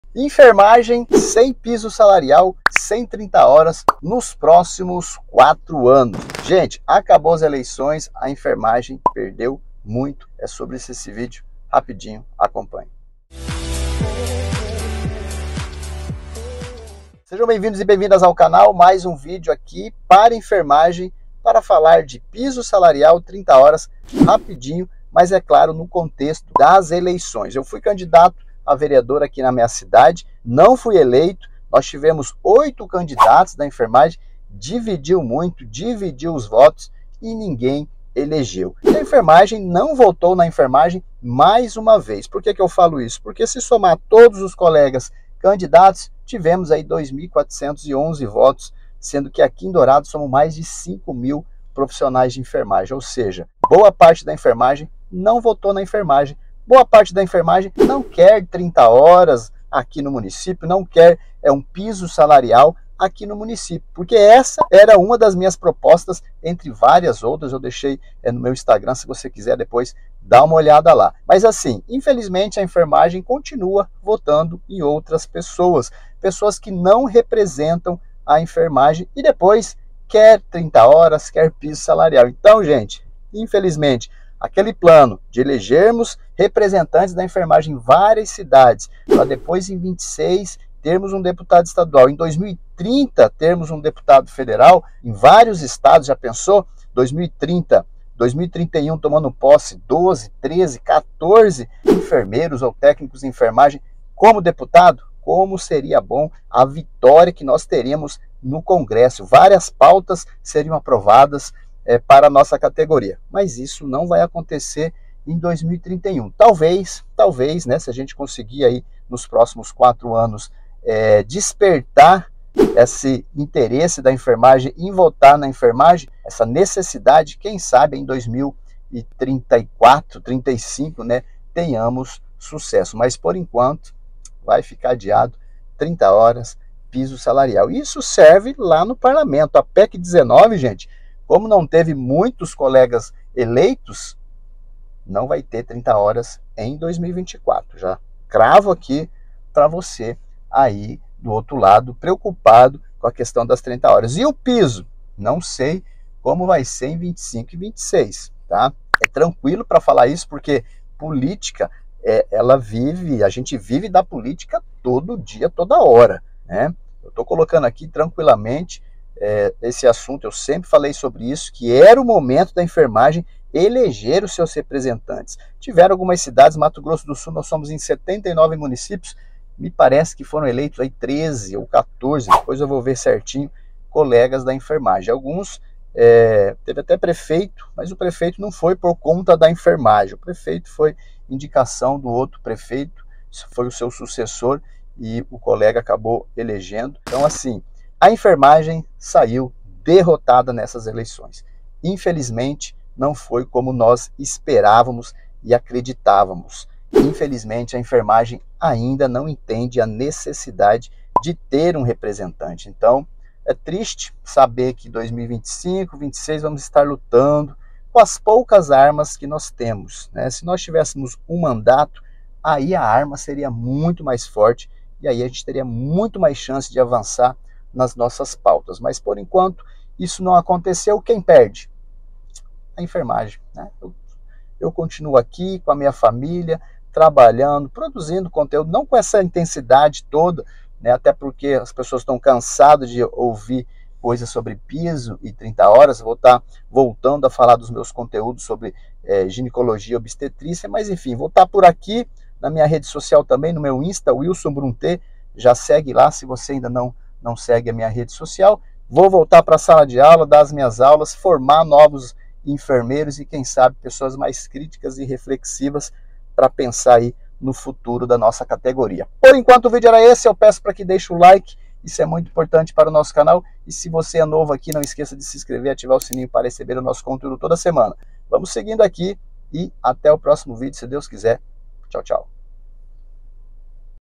Enfermagem vai ficar SEM PISO SALARIAL SEM 30 horas nos próximos 4 anos em várias partes do Brasil
As principais pautas da Enfermagem, Piso Salarial, 30 horas como jornada de trabalho, descanso digno, dimensionamento de pessoal dificilmente serão aprovadas nos próximos anos! Entenda